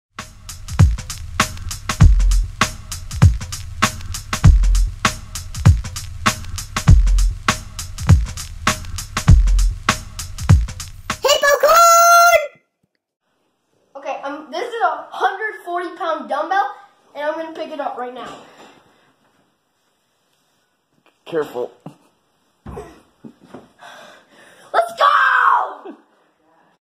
Lifting a 140 pound dumbbell stronger than my mom
140 pound dumbbell dad made it tonight.